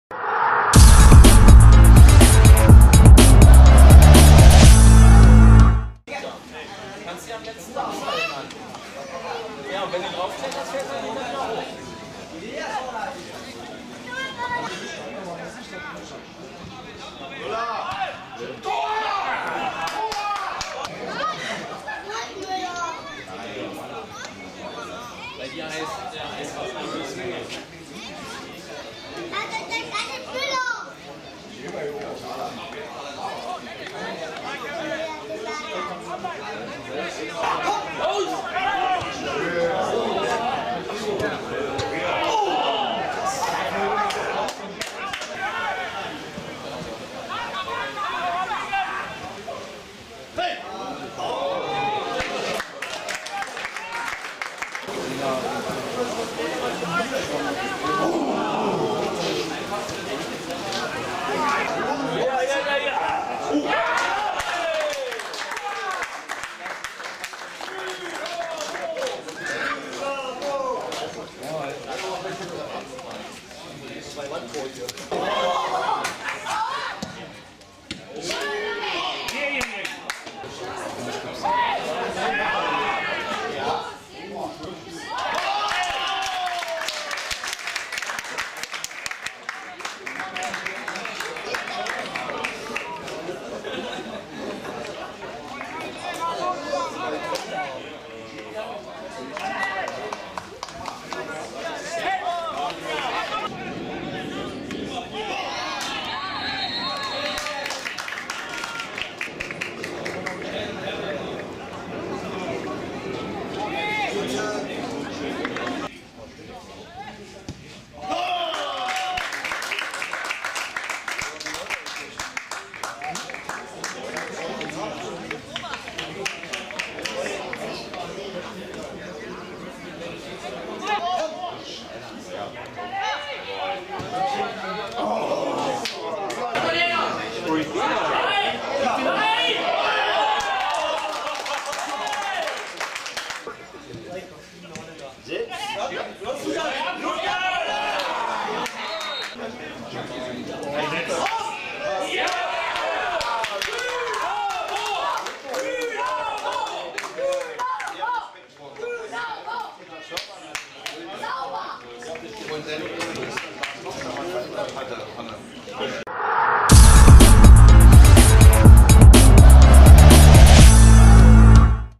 BFC Dynamo - Myanmar U19 (Testspiel) - Spielszenen | SPREEKICK.TV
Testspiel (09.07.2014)

BFC Dynamo - Myanmar U19

Tore:
1:0 Andis Shala
2:0 Christian Preiß
2:1 Maung Maung Soe
2:2 Nanda Kyaw
3:2 Tobias Scharlau